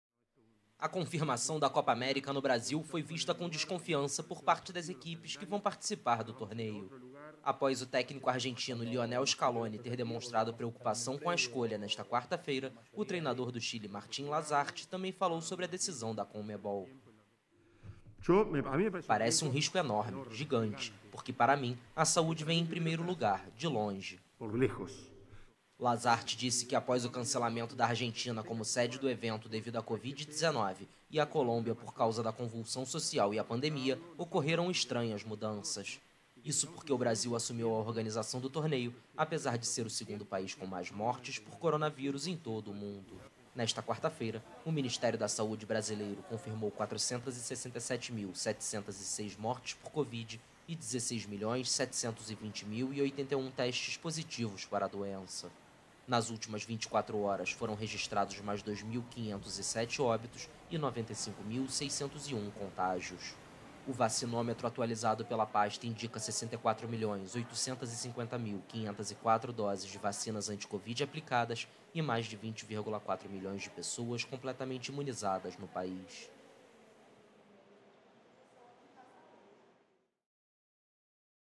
Lasarte: 'risco enorme' jogar a Copa América no Brasil
O técnico da seleção chilena de futebol, Martín Lasarte, considerou nesta quarta-feira que é um 'enorme risco' disputar a Copa América no Brasil, uma das nações mais afetadas mundialmente pela covid-19. De acordo com o Ministério da saúde brasileiro, desde o início da pandemia, mais de 467 mil pessoas morreram por coronavírus no país.